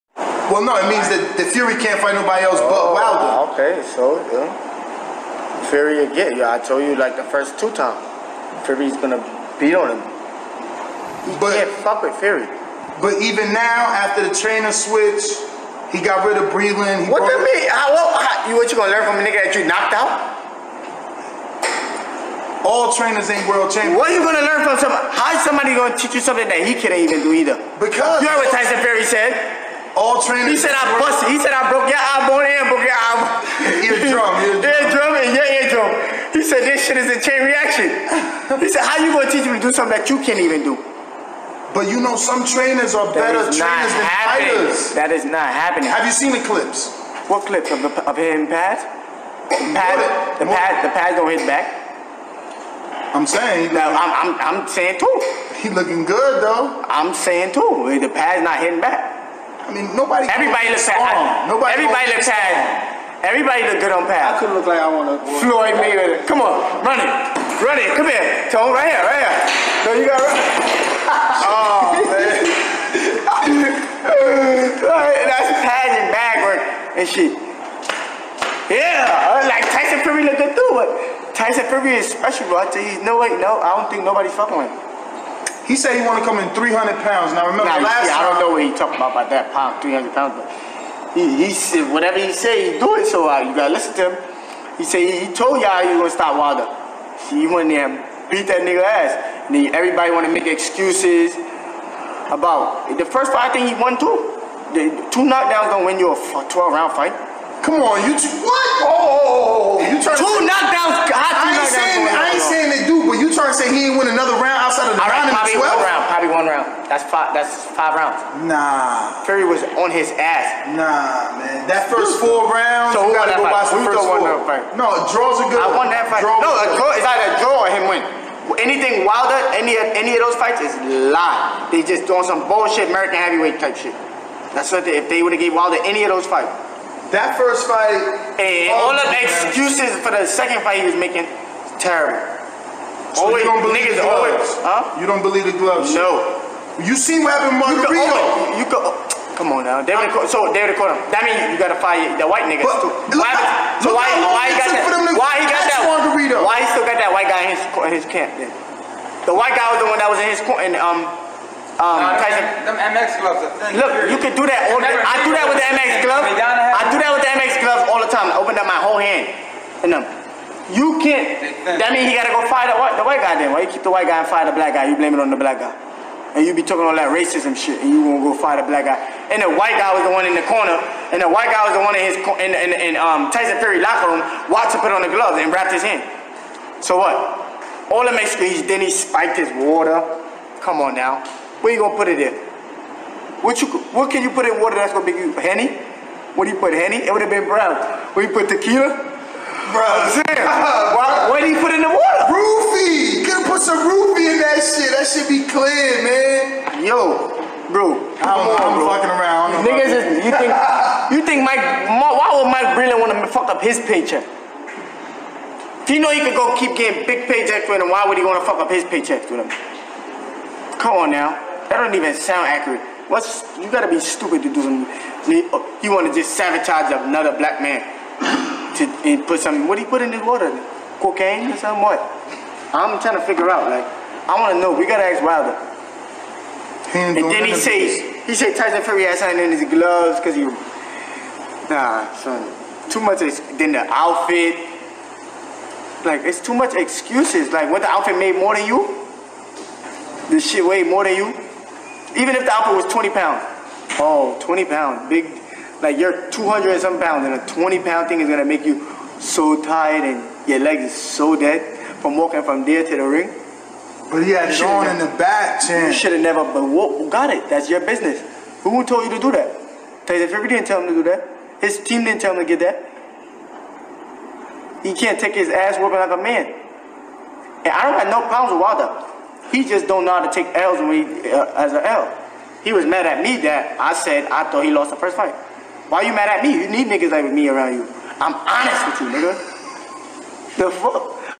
The INFAMOUS Chris Colbert Interview (with Nestor Gibbs)
130lb. contender Chris Colbert shares his True opinion on the Deontay Wilder & Tyson Fury Trilogy...